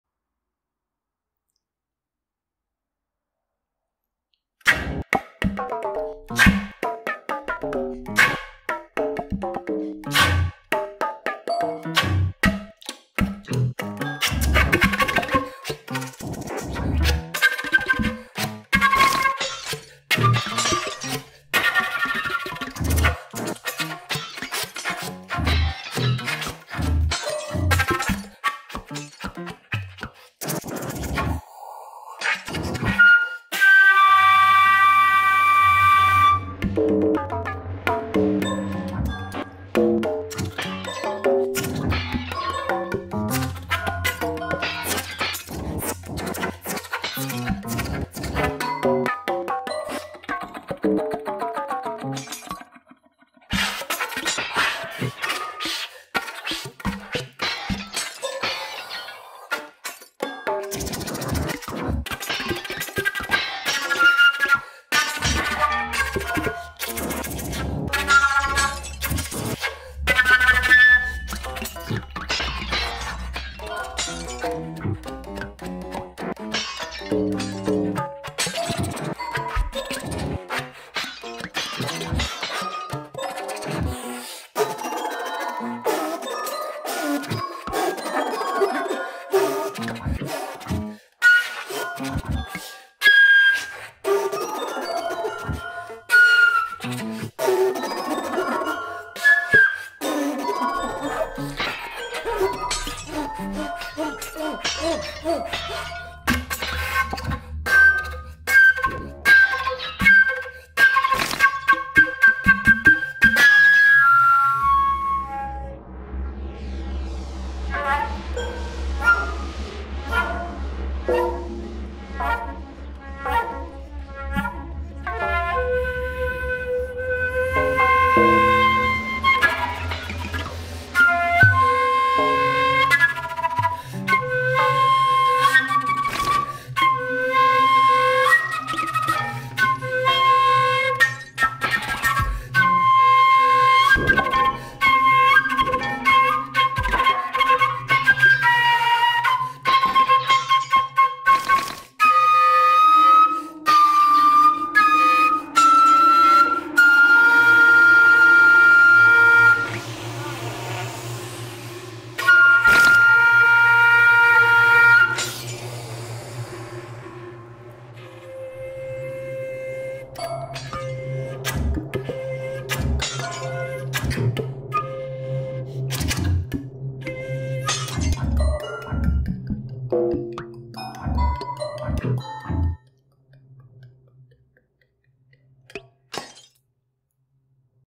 Flute and live electronic improv with Flucoma and SP-Tools
Showing some of the efforts with Sensory Percussion Tools for Max for Live by Rodrigo Constanzo.
And letting off steam :) I know it's over-the-top, but I am new to this so it's difficult to curb my enthusiasm.